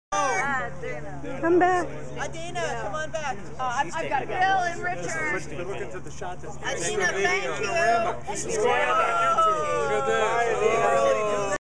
Climb to the Clouds2 CRW 2012 video
Eli Post addresses the volunteers and organizers of the Climb to the Clouds Century 101-mile ride up Mt. Watchusett. Charles River Wheelmen and women.

Mike Hanauer